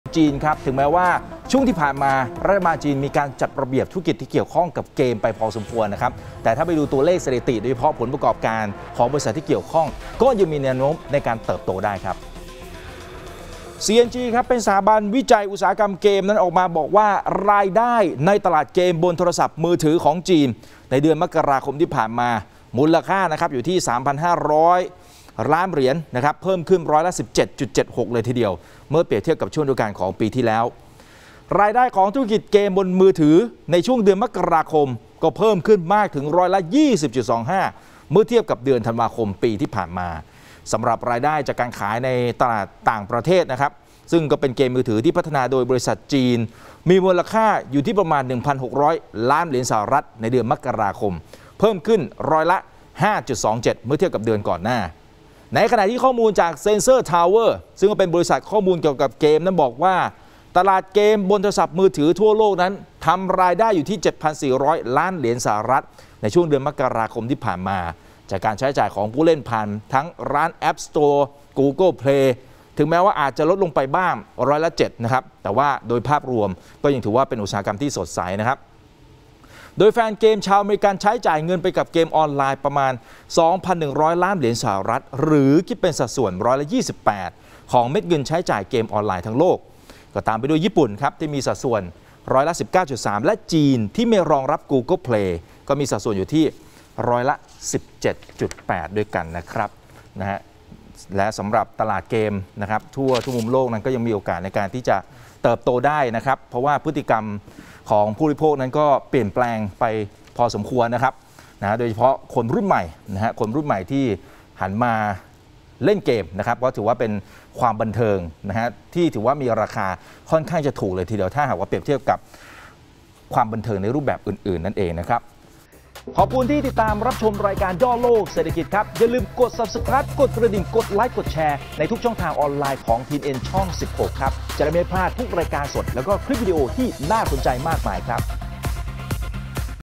ตลาดเกมมือถือในจีน ม.ค. ทำรายได้โตร้อยละ 17.76 | ย่อโลกเศรษฐกิจ 21ก.พ.65
ตลาดเกมบนมือถือของจีน เดือนมกราคม ทำรายได้เพิ่มขึ้นร้อยละ 17.76 จากช่วงเดียวกันของปีก่อน